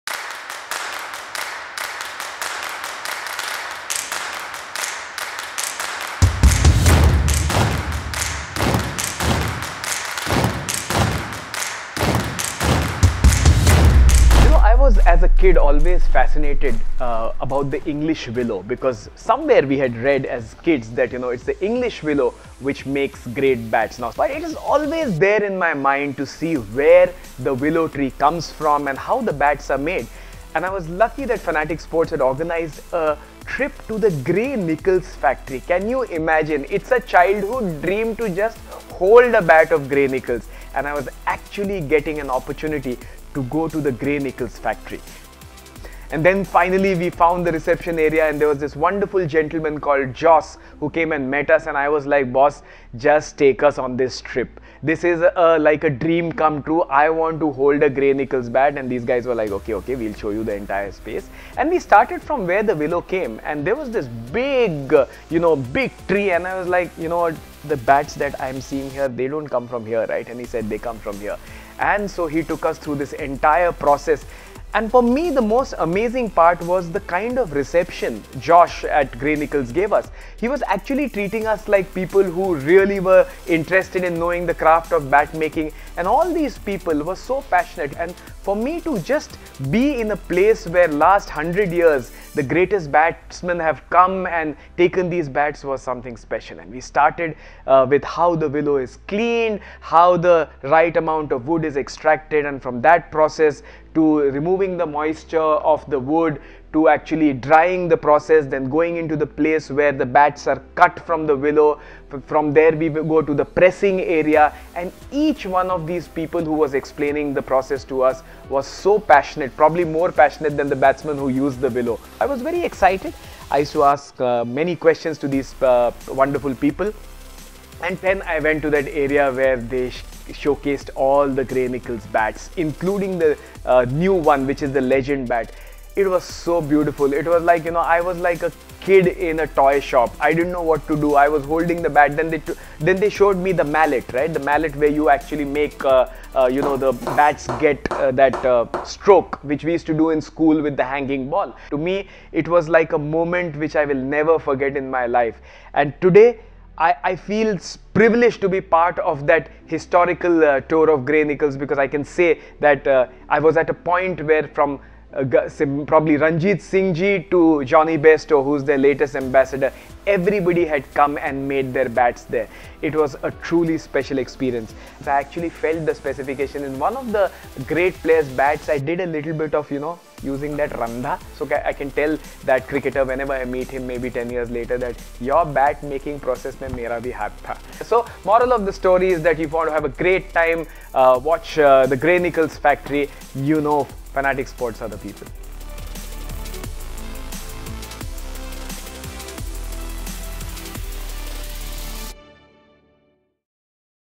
Diaries Of A Fanatic | Gray Nicolls Factory Tour ft. Vikram Sathaye
Owning a Gray-Nicolls bat is indeed special, but visiting the historic factory and watching how it's all made? Priceless!

Watch as Vikram Sathaye goes on an exclusive tour of the Gray-Nicolls factory and learns about the nuances of bat-making and more!